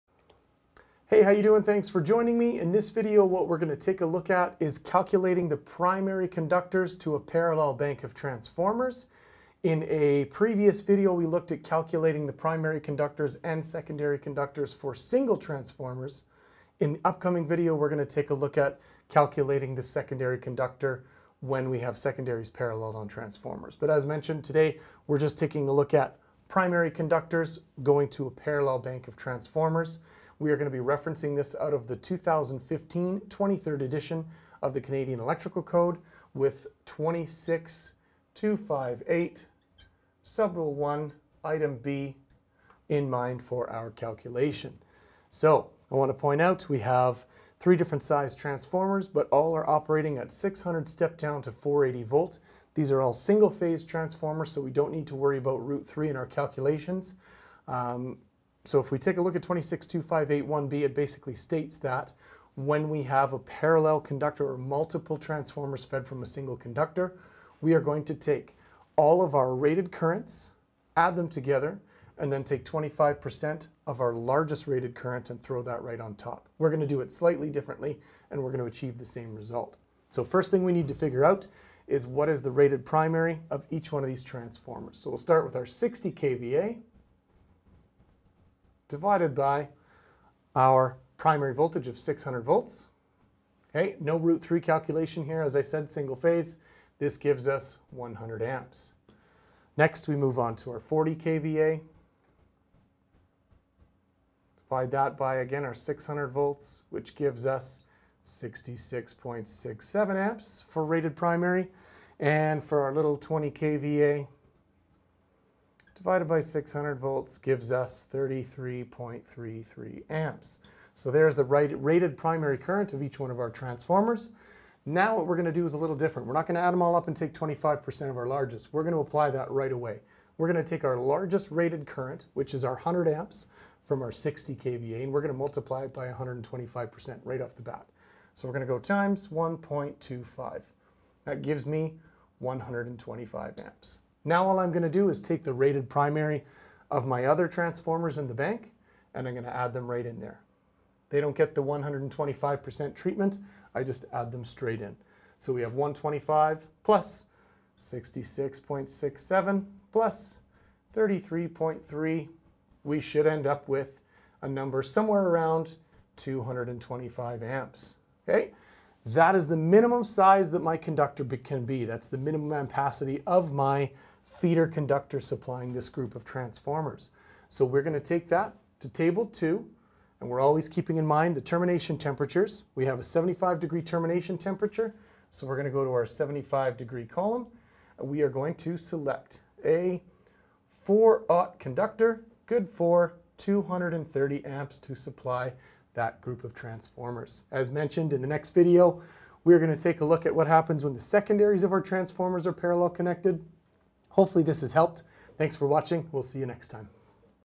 Primary Parallel Conductors (Transformers)
In this video we run through the sizing of primary conductors to a parallel bank of transformers, as required by the 2015 23rd edition of the Canadian Electrical Code. This video is meant as supplementary instructional  material for academic purposes and in no way intended to be a field installation guide.